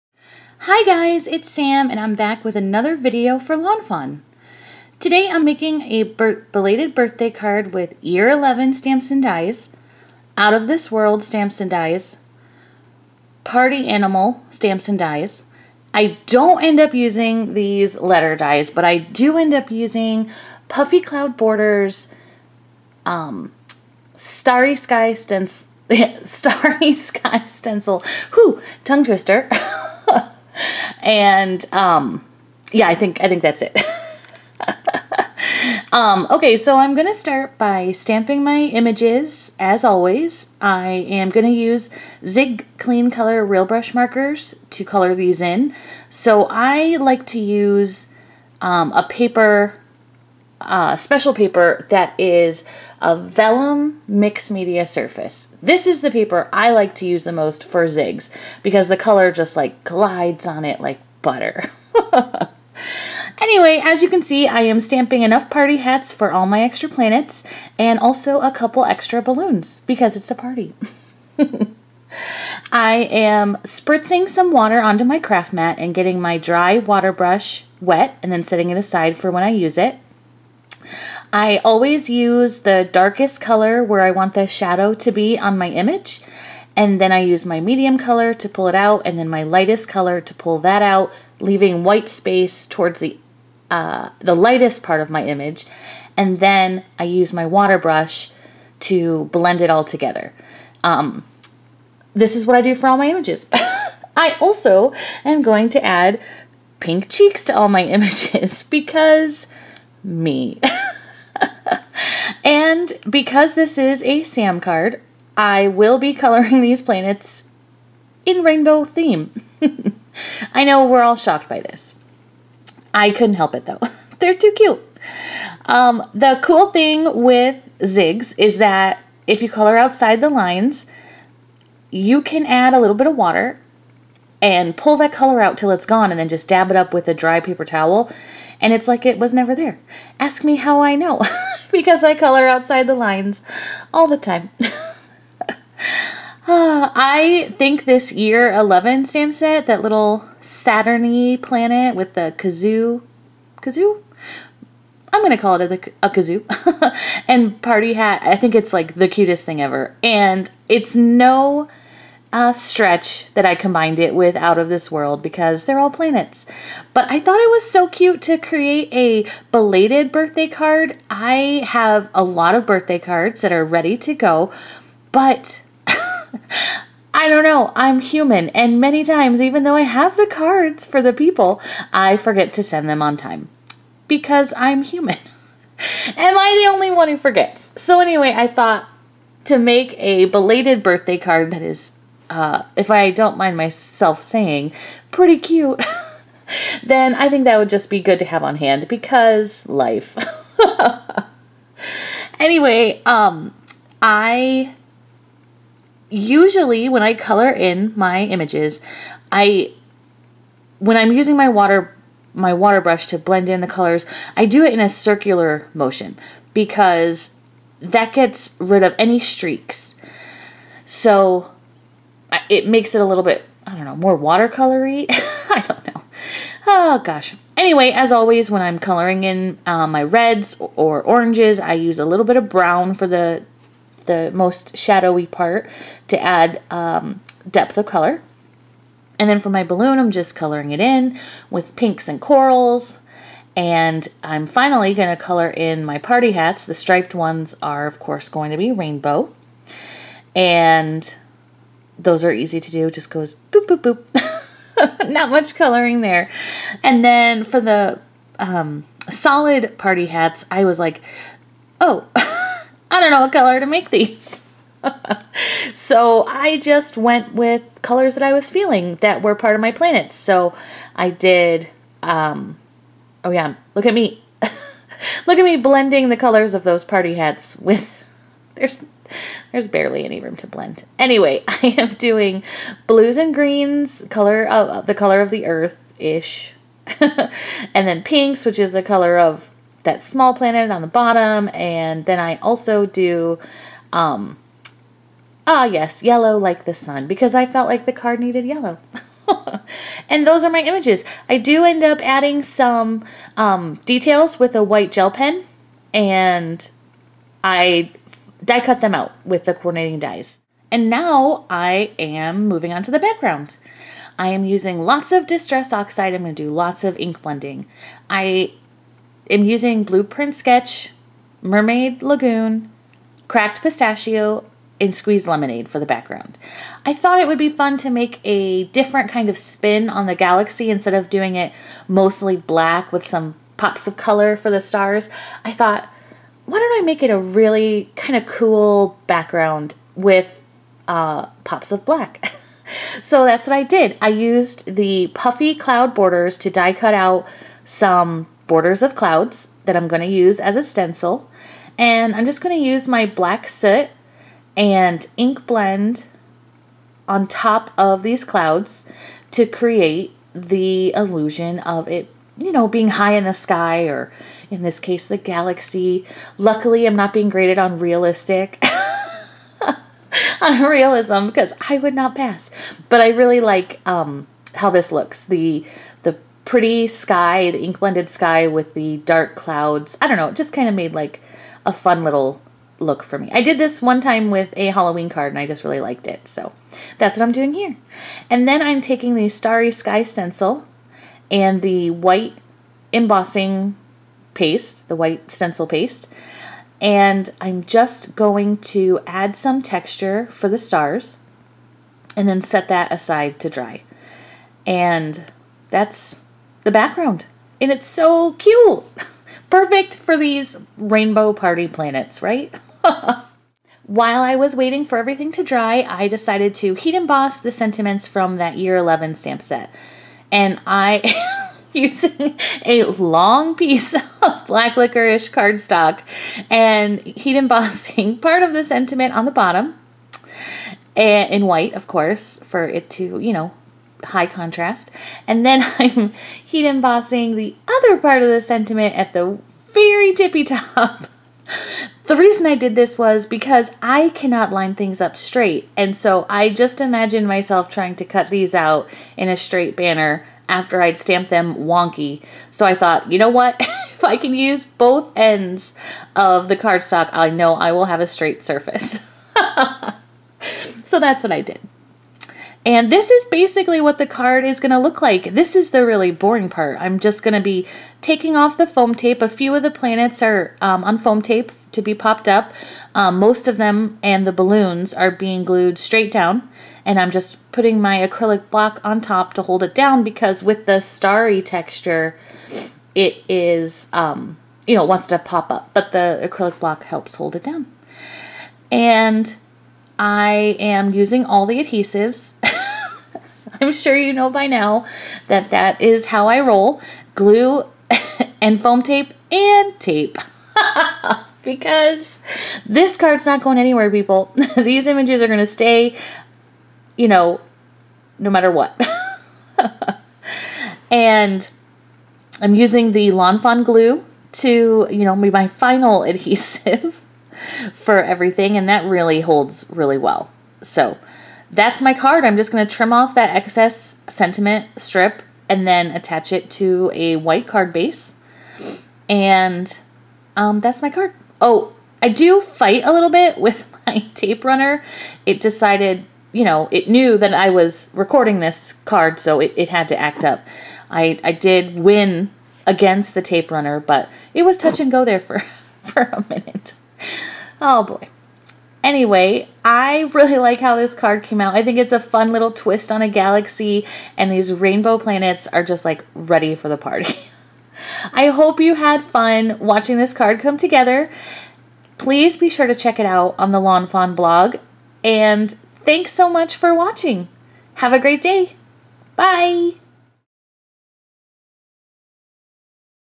Lawn Fawn Starry Sky with Clouds Belated Birthday Card
Join me as I create a cute and fun belated birthday card with Lawn Fawn products!

This card is featured on the Lawn Fawn blog, so be sure to check it out there for close ups and other fun details!

For more information and photos, head to my blog!

Zig Clean Clear Real Brush Markers

Distress Inks: Blueprint Sketch, Mermaid Lagoon, Cracked Pistachio, Squeezed Lemonade and Black Soot